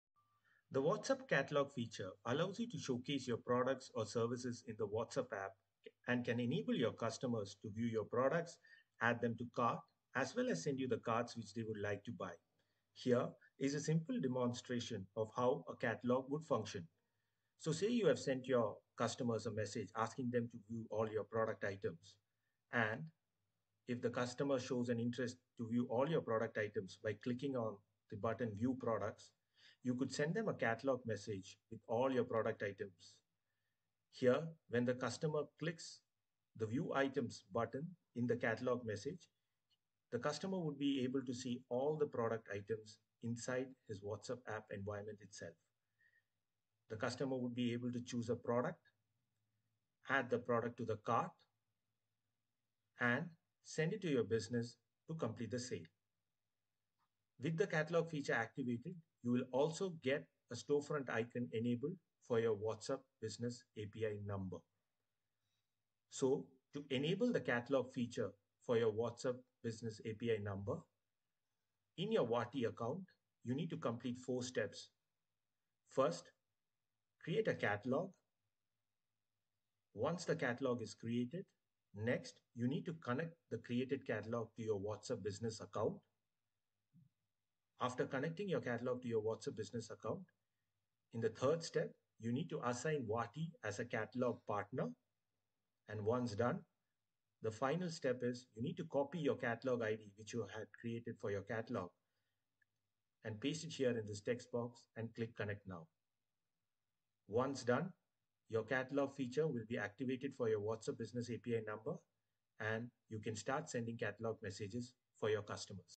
What is WhatsApp Catalog?
In this video, we explain everything you need to know about WhatsApp Catalog. Learn how to use this feature to showcase your products and communicate more effectively with your customers on WhatsApp. Watch now and see how you can increase your sales, improve customer experience, and save time.